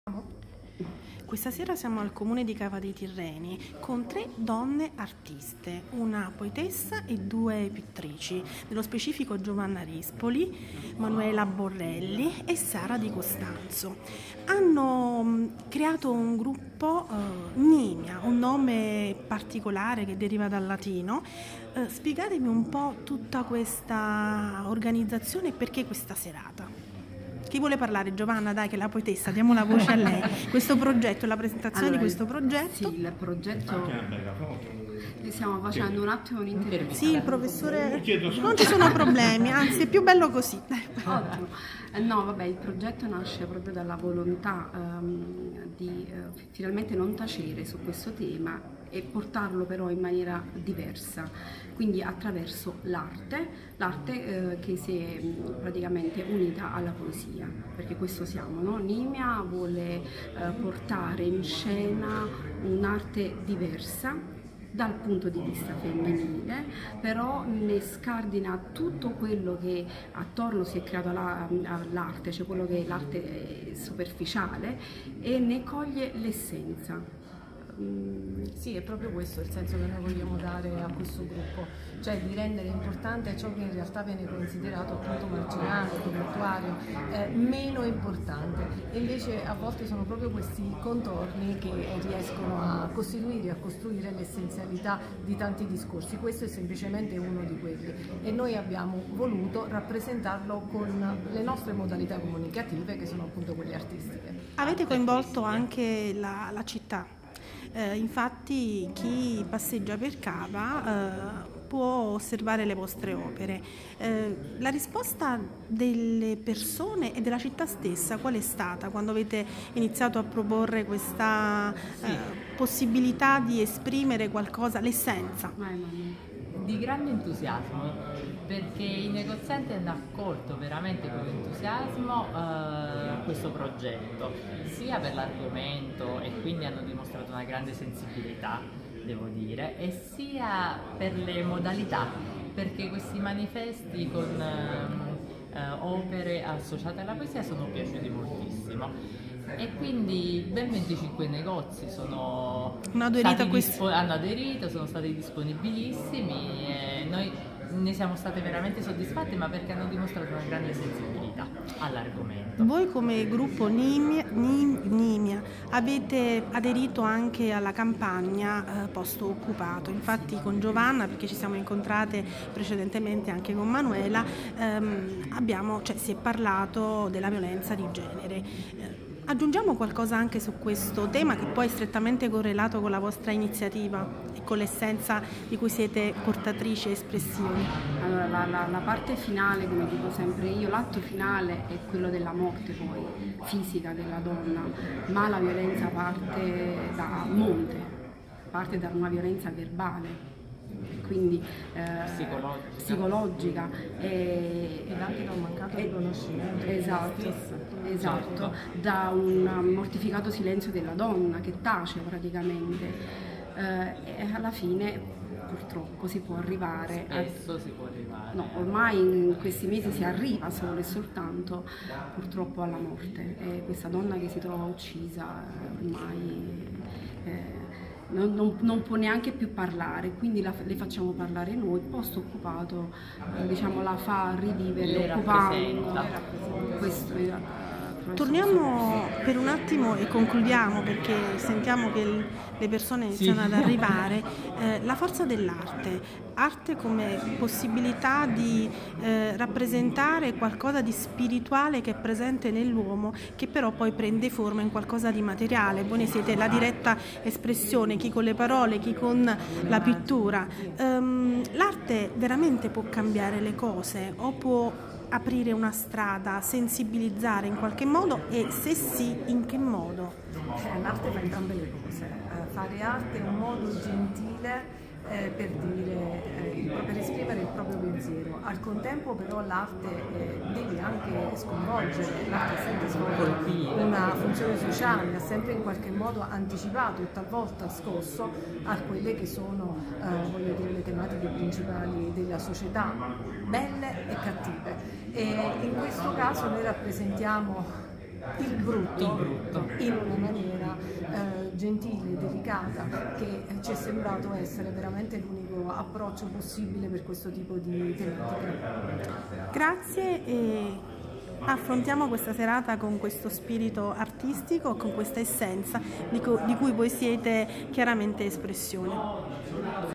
Intervista al gruppo NIMIA per il progetto "Io non ti assolvo".
Manuela Borrelli, Sara Di Costanzo e Giovanna Rispoli sono tre artiste che hanno fondato il gruppo NIMIA (termine che deriva dal latino nimius e che si traduce con le parole esagerato, potente, smodato) con l'obiettivo di trasformare in essenza il superfluo, il marginale.
Due pittrici e una poetessa che hanno pensato di creare un percorso con l'ausilio dei commercianti presenti sul territorio cavese (ben venticinque attività commerciali hanno aderito all'iniziativa) attraverso il quale mostrare le proprie opere che raccontano il disagio e la sofferenza delle donne vittime della violenza di genere che spesso sfocia nel femminicidio.
L' Arte, come sostenevano i Romantici consente di esprimere la spiritualità dell'uomo, è l'espressione della bellezza che riesce a realizzare la fusione tra corpo e spirito.